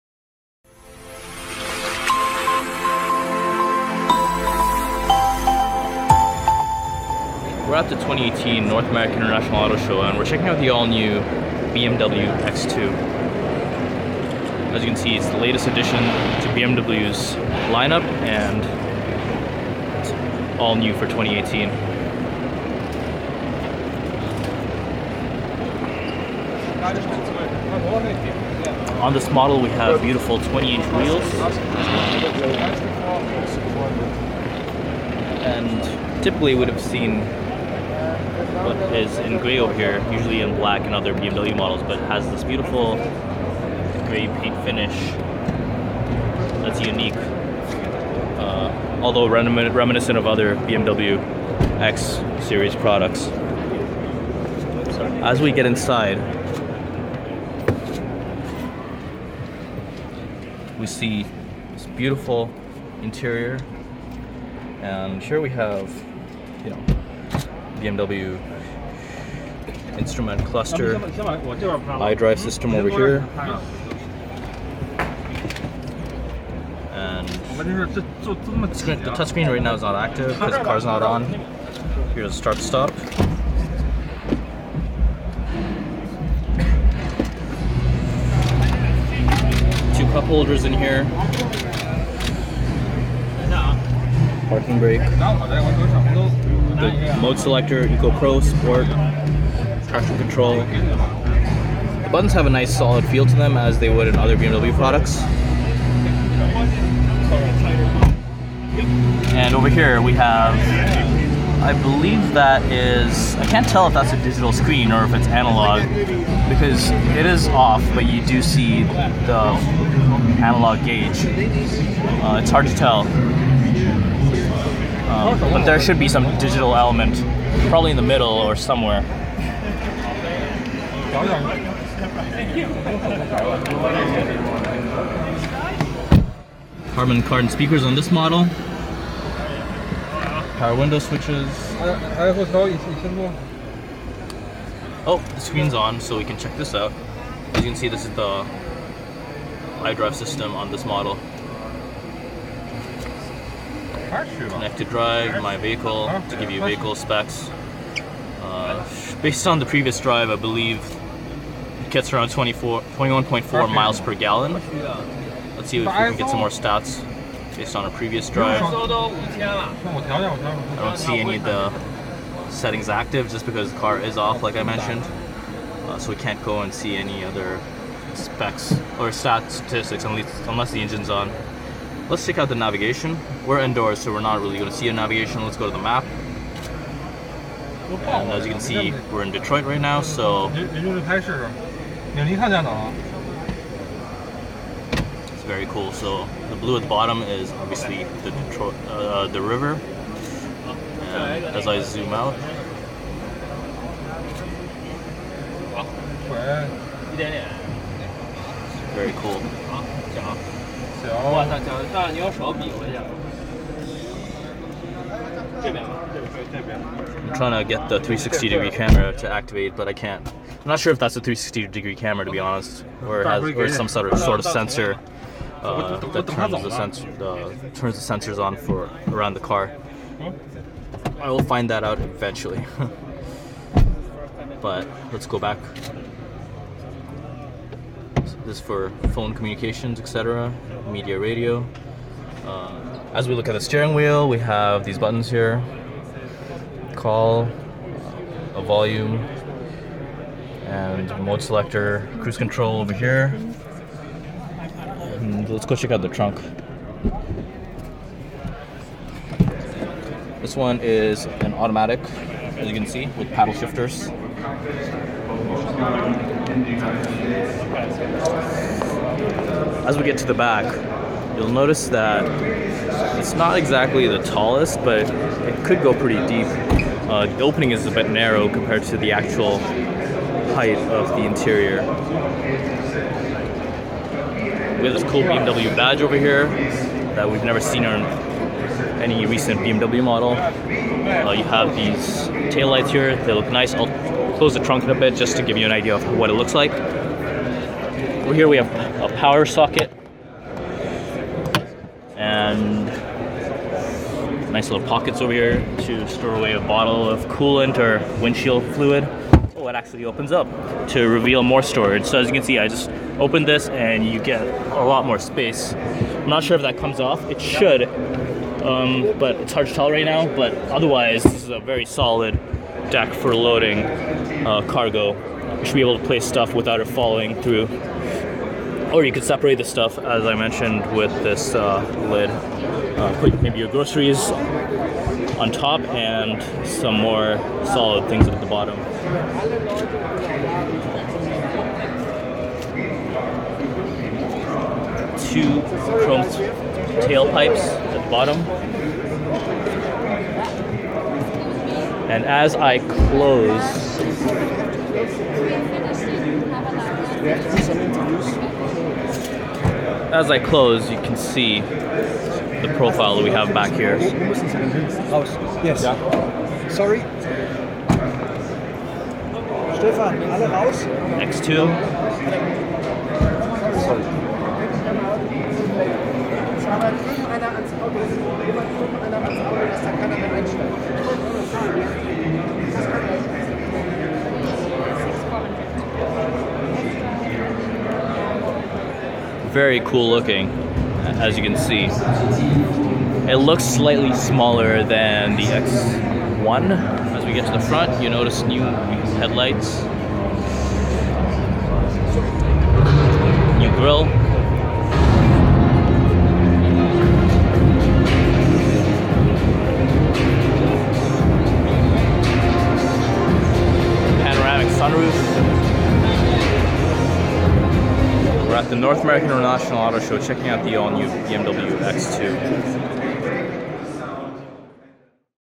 2018 BMW X2 - Walkaround, Features & Specifications
This is a look at the 2018 BMW X2 at the North American International Auto Show in Detroit.

The X2's closest competitors are:
-  Mercedes GLA
- Lexus NX
- Infiniti QX30
- Lincoln MKC
- Volvo XC40
- Audi Q3